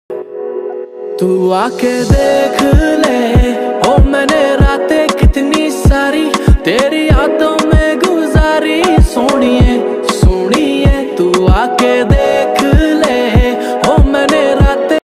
NCL mine dudhichua project surface mining, coal mine NCL, CIL, hemm machine Dragline in singrauli
Jyoti dragline at dudhichua